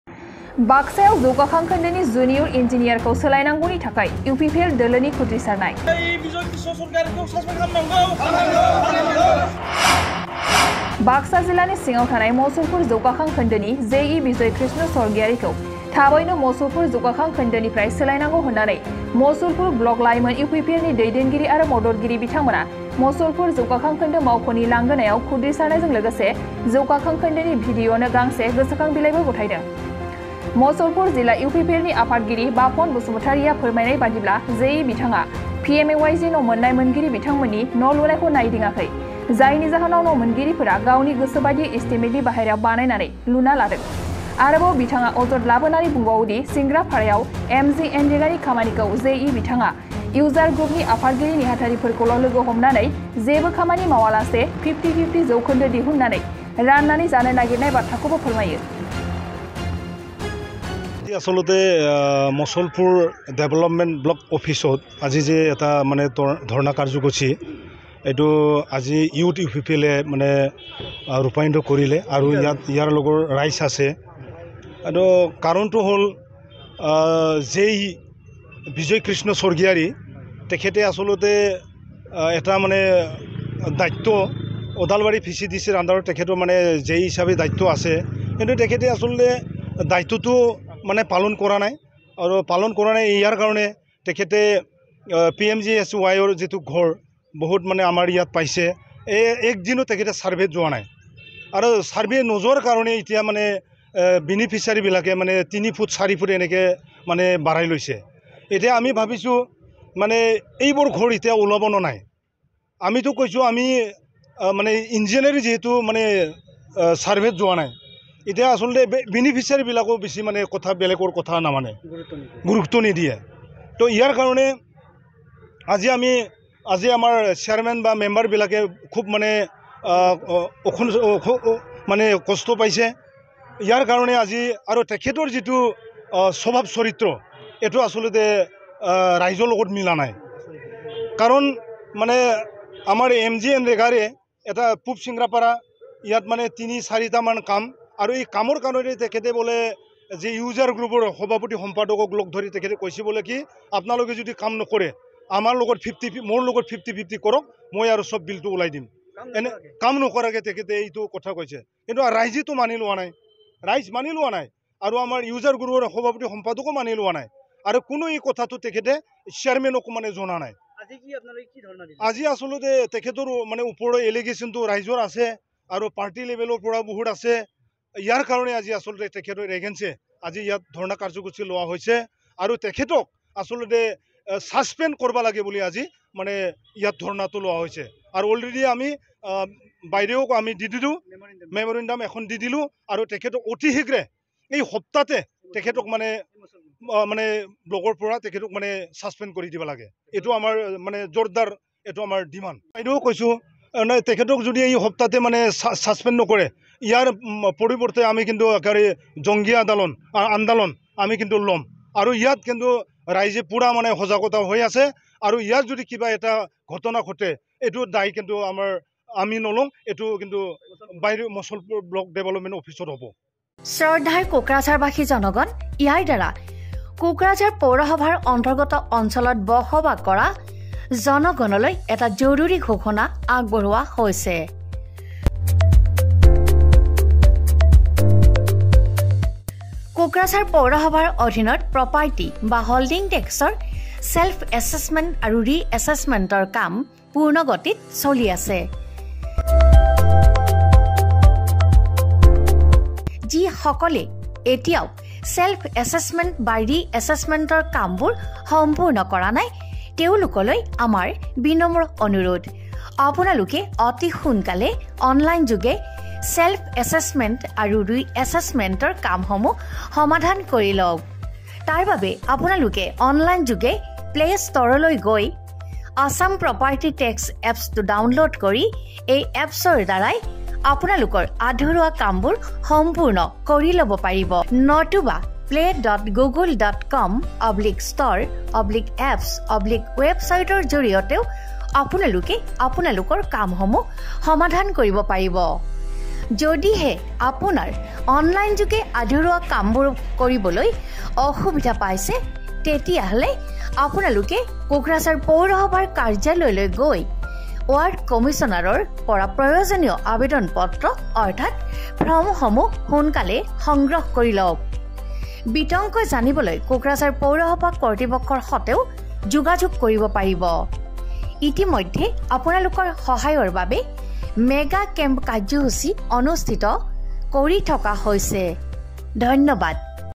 बाक्सायाव जौगाखां खोन्दोनि जुनियर इनजिनियार खौ सोलायनांगौनि थाखाय UPPL दोलोआ खुद्रिसारदों
बाक्सायाव  UPPL दोलोआ खुद्रिसारदों